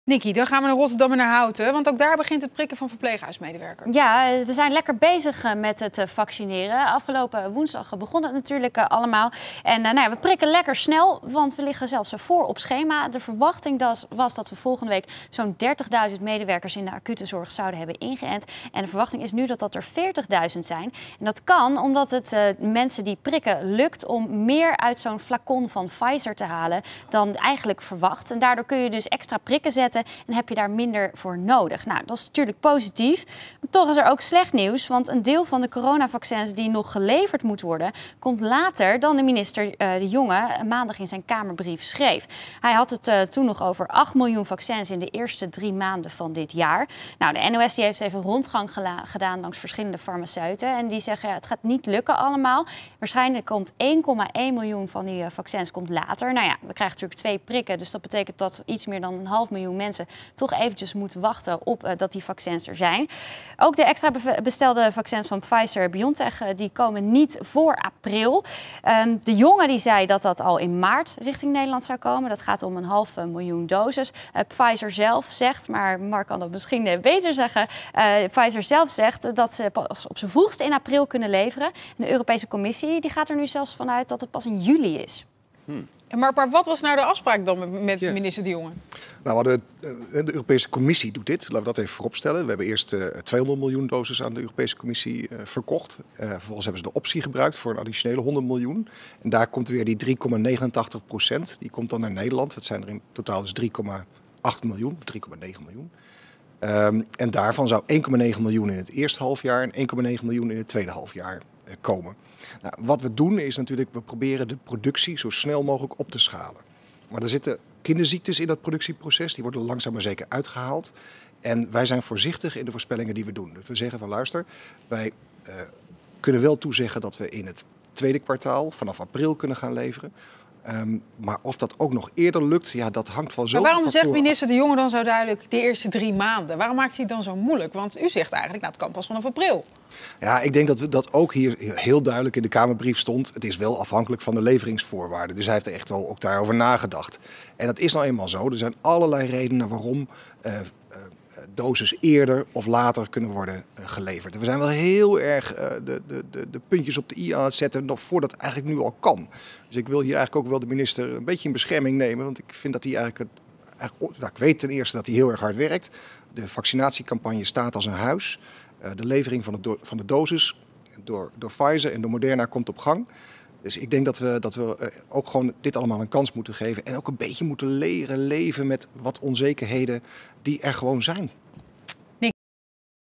Latere levering vaccins: ‘Kinderziektes worden er langzaam uitgehaald’
In Houten en Rotterdam begint vandaag het vaccineren van verpleeghuismedewerkers. Nadat afgelopen woensdag het startschot werd gegeven, ligt het vaccinatieplan voor op schema.